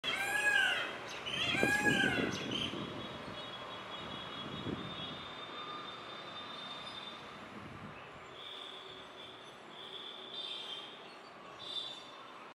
Beautiful Indian peacock in my backyard during monsoon season rains 🦚.
Here’s a short video of a beautiful Indian peacock in my backyard. It’s common to spot many beautiful birds in my backyard during the monsoon months.